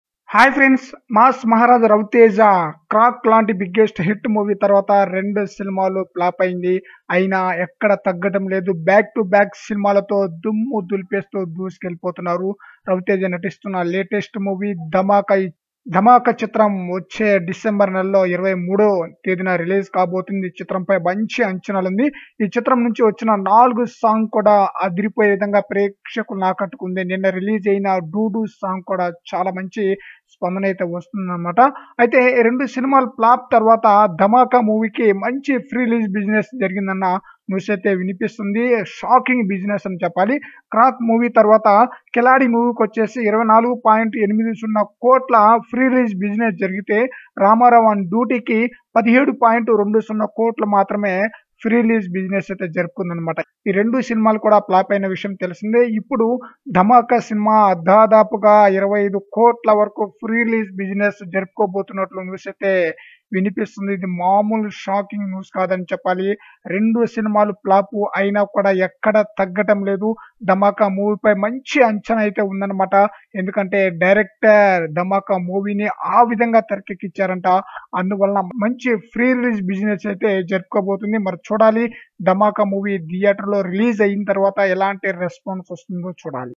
రవితేజ ధమాకా షాకింగ్ ప్రీ రిలీజ్ బిజినెస్ | Dhamaka Pre Release Business | Ravi Teja | Sreeleela
hi friends mass maharaj ravi teja natistunna latest movie dhamaka,rendu flop movie tarwata release kanunna dhamaka movie pai anchanalu tara stayilo undi,dhamaka movie pre release businees